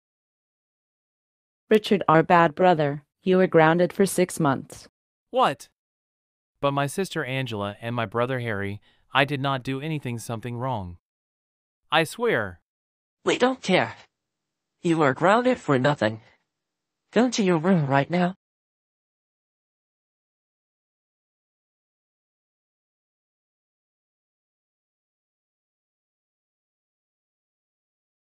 Richard M. Greenwood gets grounded for nothing
Richard M. Greenwood gets grounded by his own siblings Angela and Harry.

This is the first grounded episode of Richard M. Greenwood.

Cast of voices:
Richard M. Greenwood - Microsoft Azure Jason/Voicemaker Jayden (🇺🇸)
Angela Greenwood - CereProc Isabella (🇺🇸)
Harry Greenwood - Evan from Koe.ai